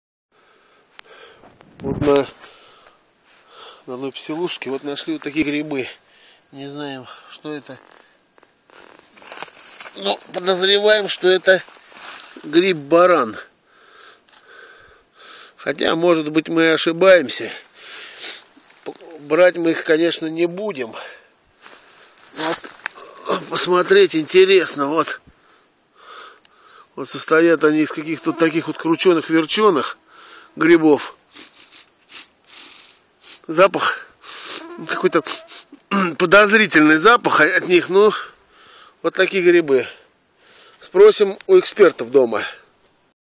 Лесной баран
гриб баран найден в Солнечногорском районе